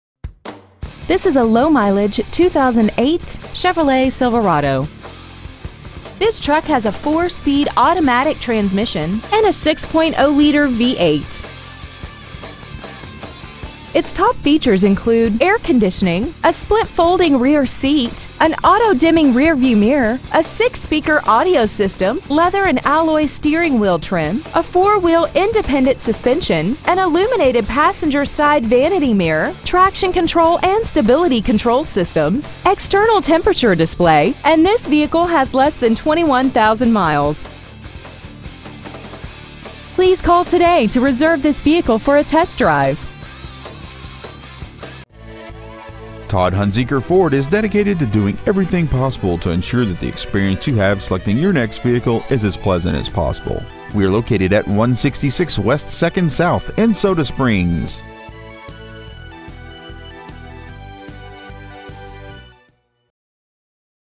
2008 CHEVROLET SILVERADO 1500 ID
We have been honored to serve the  ID area , we promise that your experience at our dealership will exceed your expectations !

 Year : 2008
 Make : CHEVROLET
 Model : SILVERADO 1500
 Engine : 6.0L V8
 Trans . : AUTO 4SPD
 Exterior : DESERT BROWN
 Miles : 20,967

 Stock : F11U233

 166 West 2nd South
  , ID 83276